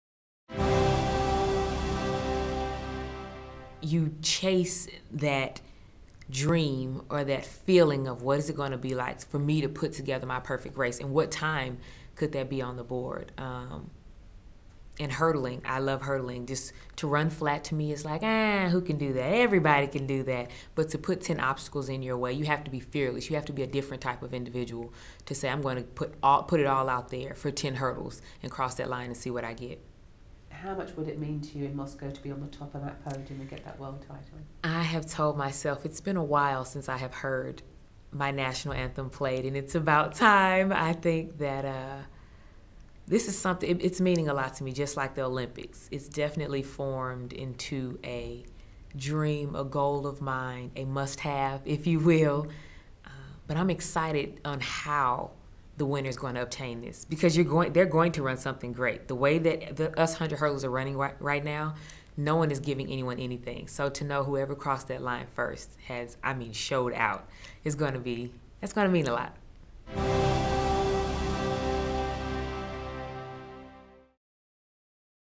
WCH Moscow 2013 - Dawn Harper - Preview
Dawn Harper is looking forward to the IAAF World Championships in Moscow 2013